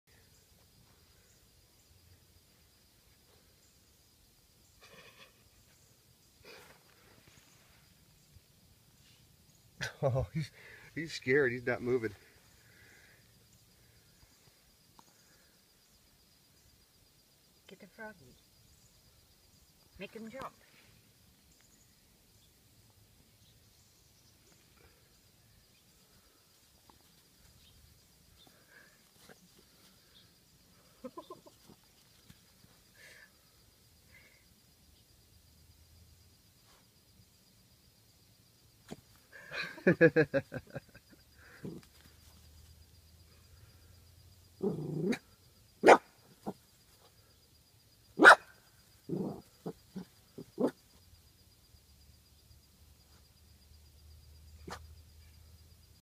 Max the Dachshund sees his first frog.
Most of the tree frogs around here are the tiny ones. We came across one of the larger species today.